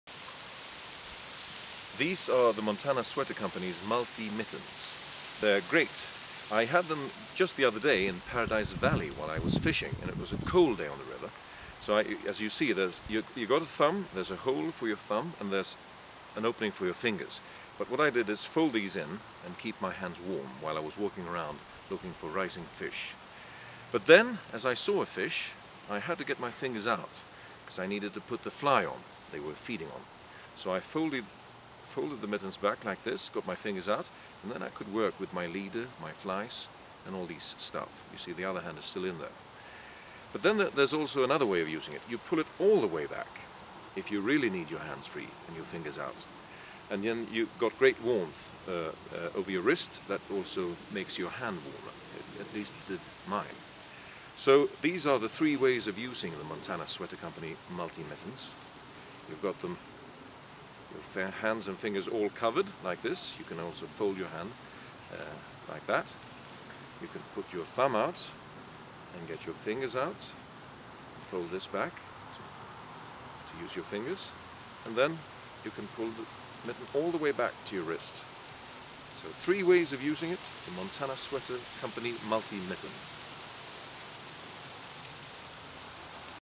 The Bison Multi-Mitt
Our original design, knit for fly fishers and those who need more mitt when nature has turned cold.  Field tested in the rugged west, the Bison Multi-Mitt is warm and versatile.  Knit with our popular, Bison Ranch Yarn.